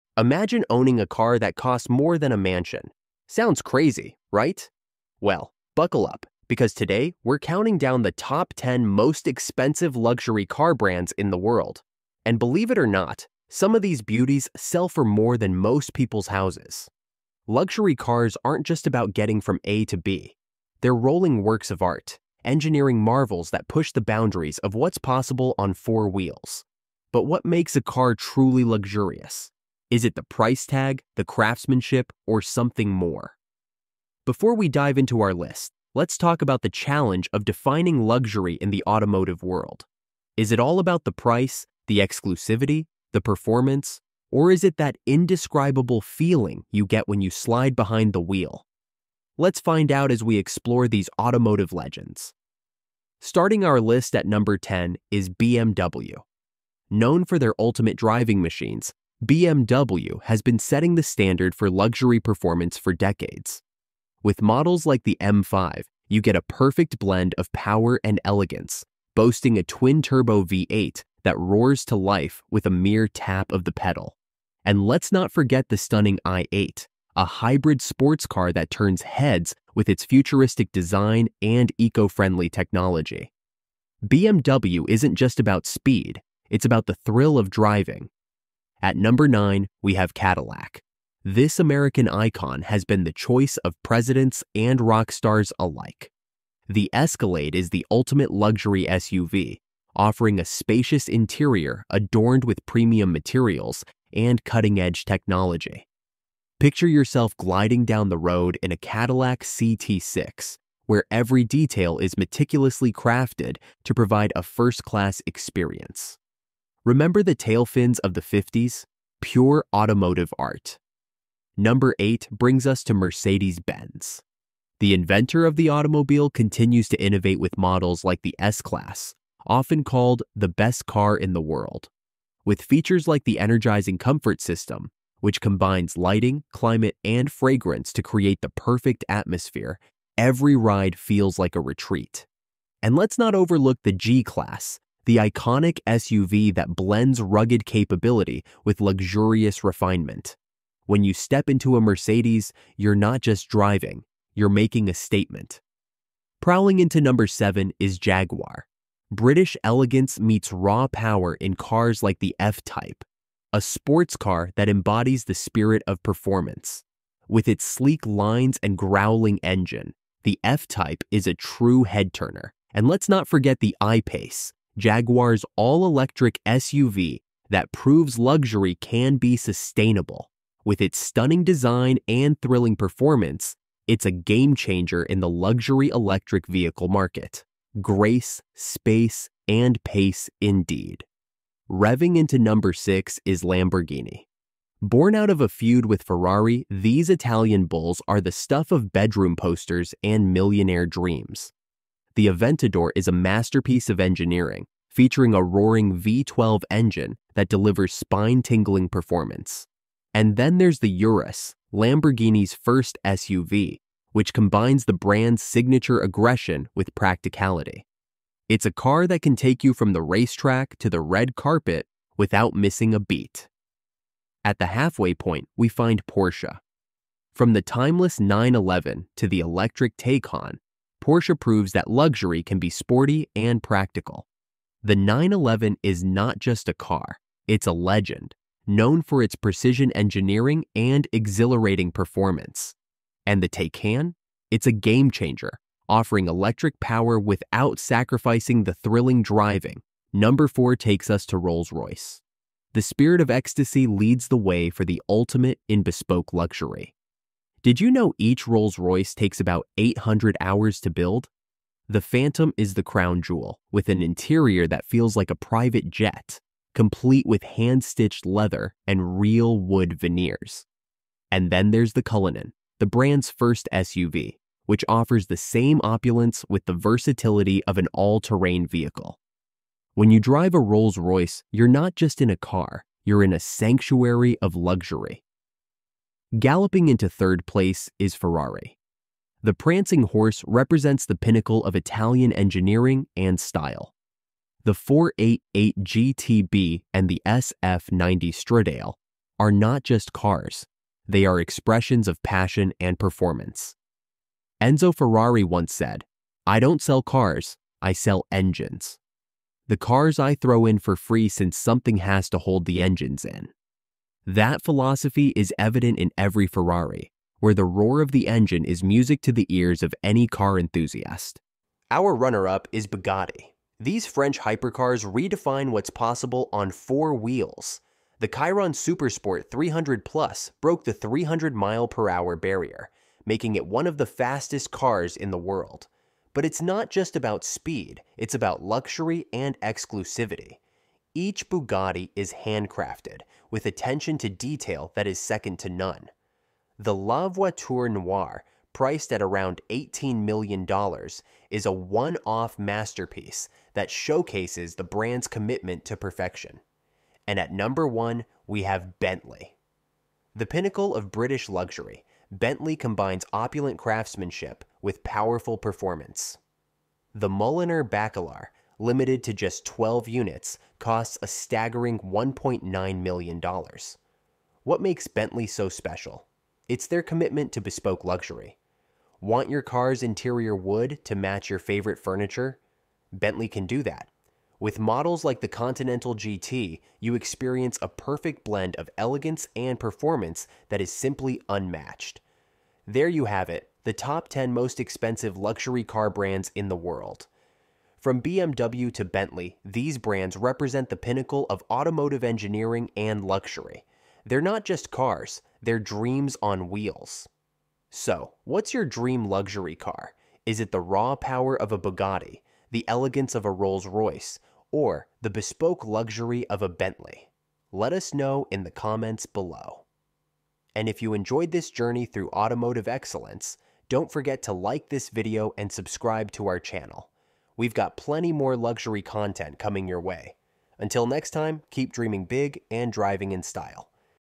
Top 10 Most Expensive Luxury Car Brands in the World 2024
In this video, we explore the **Top 10 Most Expensive Luxury Car Brands in the World for 2024**! Join us as we take a closer look at the most prestigious automotive manufacturers that define opulence and performance. From iconic brands known for their unparalleled craftsmanship to innovative companies pushing the boundaries of technology, discover which luxury car brands top the charts this year.

- A countdown of the most luxurious car brands and their standout models.
- Insight into the unique features that make these vehicles so expensive.
- Information on the craftsmanship, technology, and heritage behind each brand.
- A glimpse into the future of luxury cars and their impact on the automotive industry.

Whether you're a car enthusiast, a luxury lifestyle aficionado, or simply curious about the world of high-end automobiles, this video is for you!